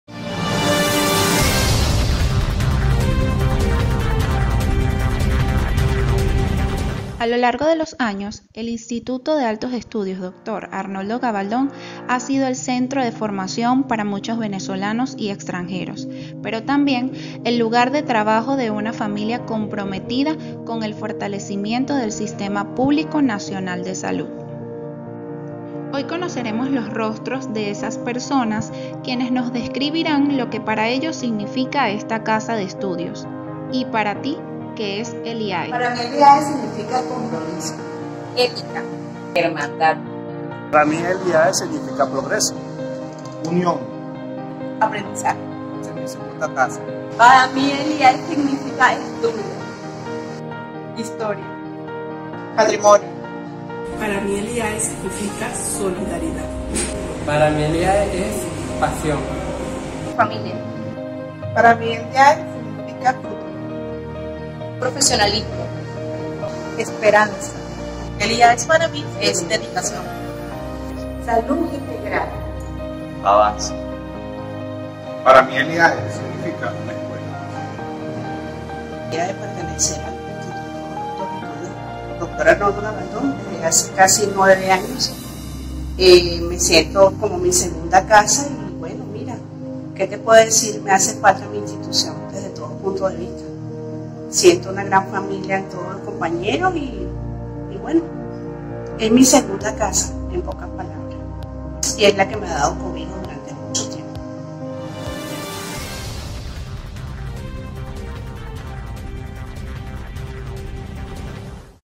Y PARA TÍ ¿QUÉ ES EL IAE?
A lo largo de los años, el Instituto de Altos Estudios Dr. Arnoldo Gabaldon, ha sido el centro de formación para muchos Venezolanos y Extranjeros. Pero también el lugar de trabajo de una familia comprometida con el fortalecimiento del Sistema Público Nacional de Salud...